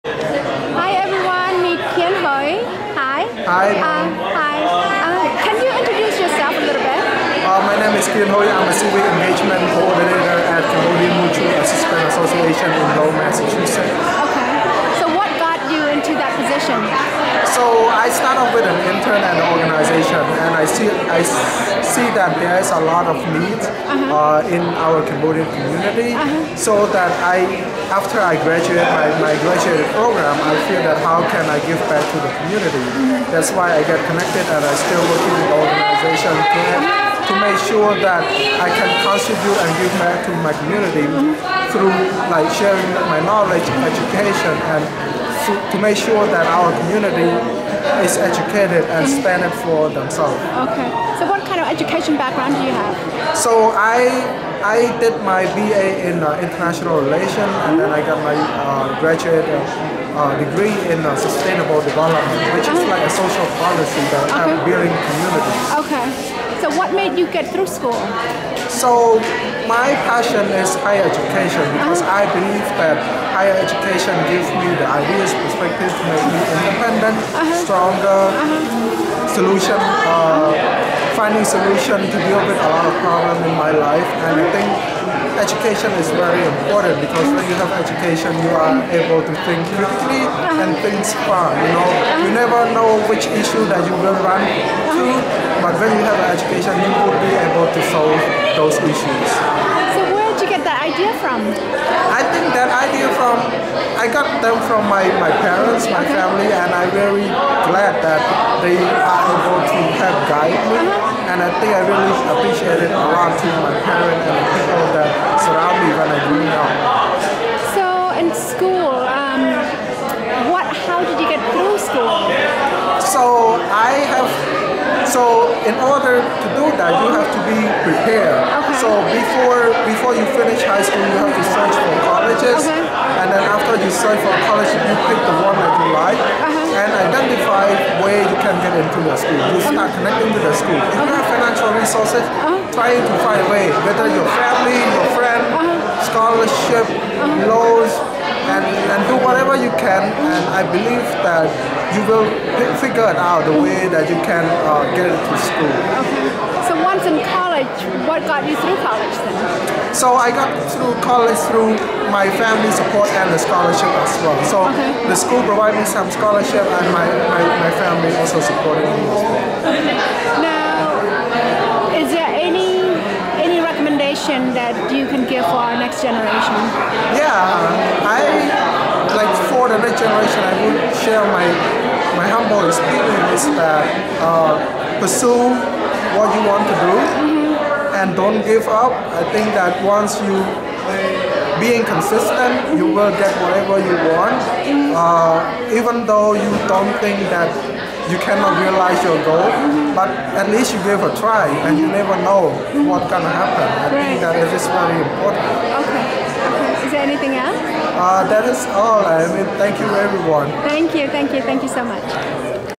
Keanhuy Sour - Civic Engagement Coordinator at CMAA in Lowell, MA.
Khmer Power at Leadership and Advocacy Training (LAT) 2019 hosted by Southeast Asia Resource Action Center (SEARAC):

SEARAC received about 200 applicants for the LAT 2019 but could only accept 48 applicants. Thirteen Khmer community leaders and/or advocates from across the U.S. were among the 48 applicants that were selected. Two Cambodian LAT alumni were called back to be the facilitators. I was one of the applicants who got accepted to be trained on June 19-21, 2019 for the following purposes:
1. To enhance the leadership skills of Southeast Asian American (SEAA) leaders
2. To build and strengthen relationships between local-level and national-level leaders.
3. To engage Southeast Asian Leaders from throughout the nation in national level community development and advocacy work.
We were brought together to build, grow, and empower a network of advocates to advance a national SEAA movement for social change under three tracks: Education, Health, and Immigration. I selected the Education Track because it is my expertise. I also believe that an educated person would generally know how to navigate to get help on health and immigration issues better than an uneducated person.
We were trained to effectively communicate the issues that we care about with supporting statistics, to build solutions from our own experiences, and to demand actions from the U.S. Congress by directly talking to them or their staff in the Capitol Hill.
For my small group in the education track, we asked the staff from House Speaker Nancy Pelosi, Senator Diane Feinstein, and Representative Anna Eshoo for a commitment to support robust funding for federal outreach and student services programs such as TRIO programs and to expand it toward SEAA communities. Overall, the staff were very supportive and were committed to recommending their bosses as well. For example, the Speaker's Legislative Correspondent, Sarah Swig, even asked for my business card and said that she would connect with my local constituent to work directly with me. I was just one of the 13 Khmer advocates in this training. These Khmer advocates have done amazing work in their local communities and now are taking their issues to the national level. I couldn't have been more proud of our Khmer community!

Marlai